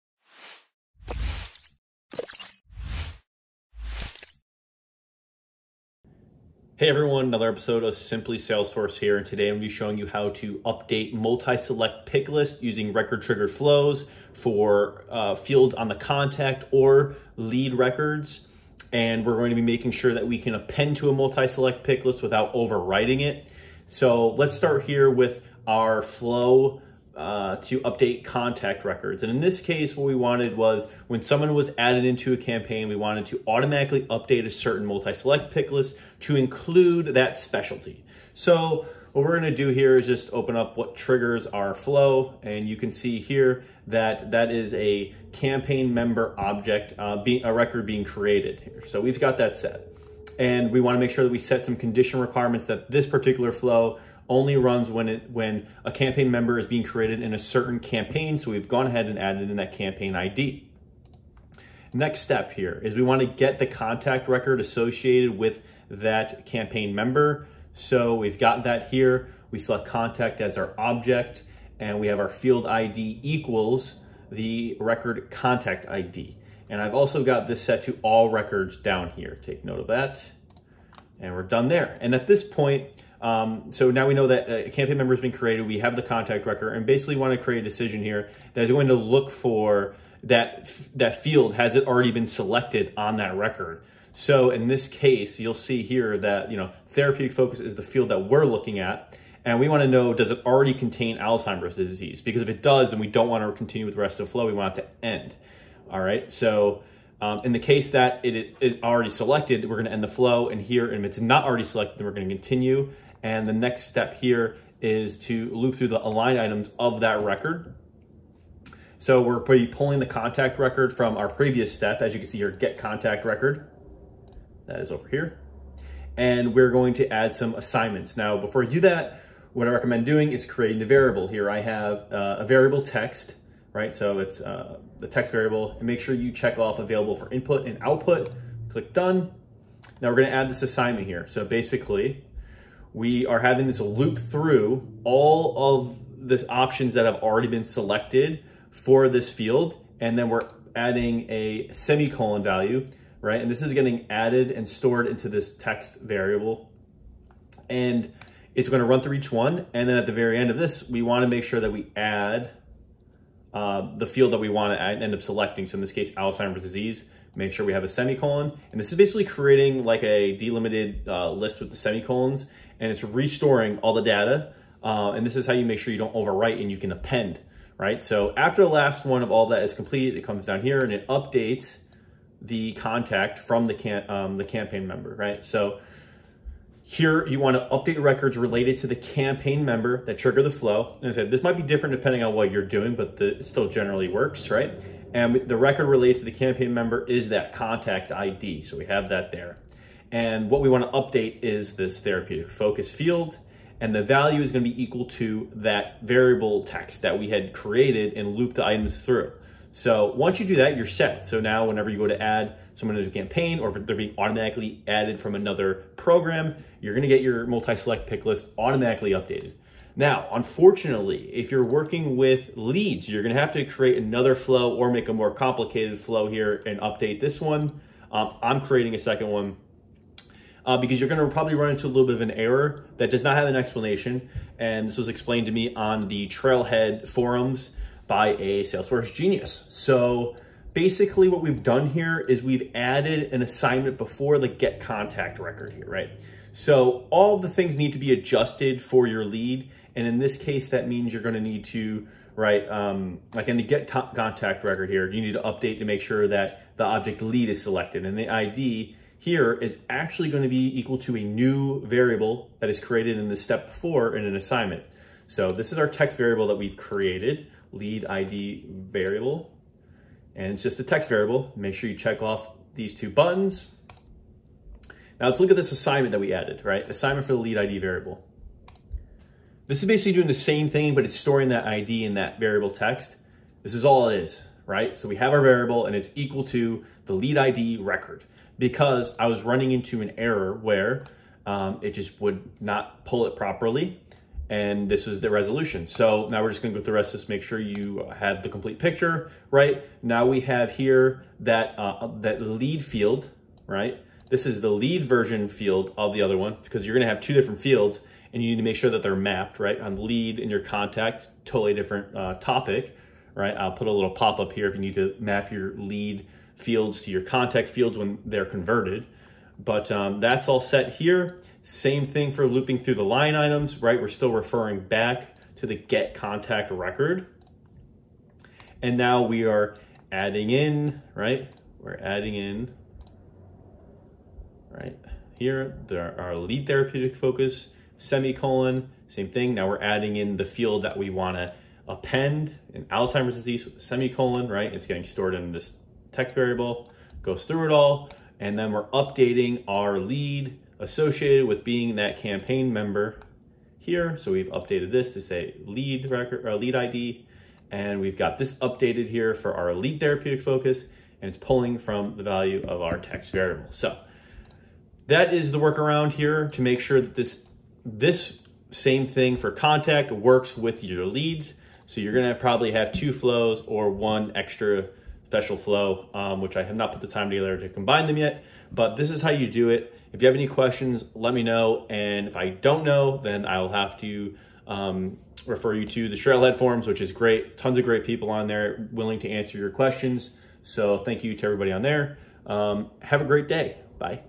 Add a Value to Multi-Select Picklists in Salesforce Flow Builder using Loop & Assignment
In this video I explain how to update/append a multi-select picklist (MSP) by using a record-triggered Flow in Salesforce on a contact or lead record. MSPs have some pitfalls and I hope in this video, you'll be able to automatically update them for your use case.

Timestamp Chapters:
00:00 Intro/Background
00:40 Update MSP for CONTACTs
03:40 Update MSP for LEADs

I highly recommend using Salesforce Trailhead for the questions forums and learning modules if you aren't already

Episode 3 of Simply Salesforce Tutorial